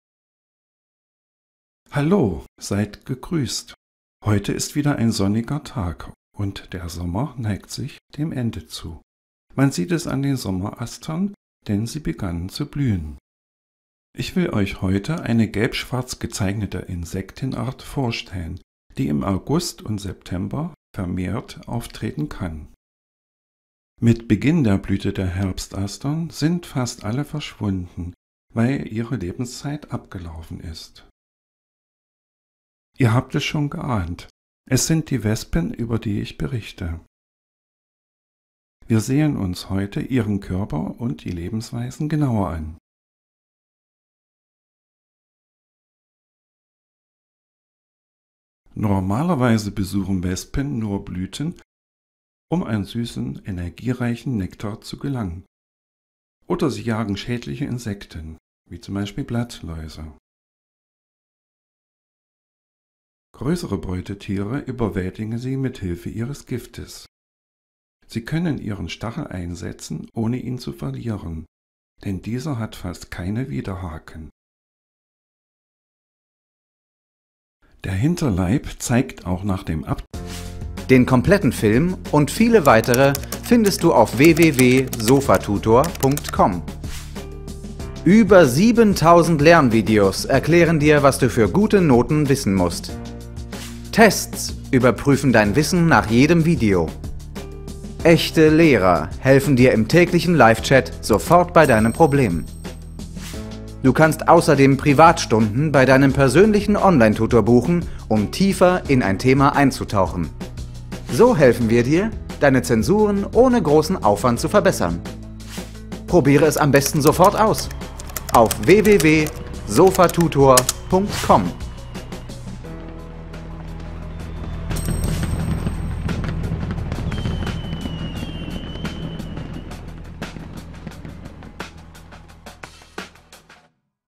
Staatenbildende Insekten am Beispiel der Wespe | Biologie | Biologie der Tiere (Mittelstufe)
Diese Video beschreibt die Wespe und ihre Staat. Du erfährst etwas über ihren Körperbau und welchen Nutzen sie für die Natur haben.

Dieses Video zum Thema Insekten gehört zu: Biologie | Biologie der Tiere (Mittelstufe) | Insekten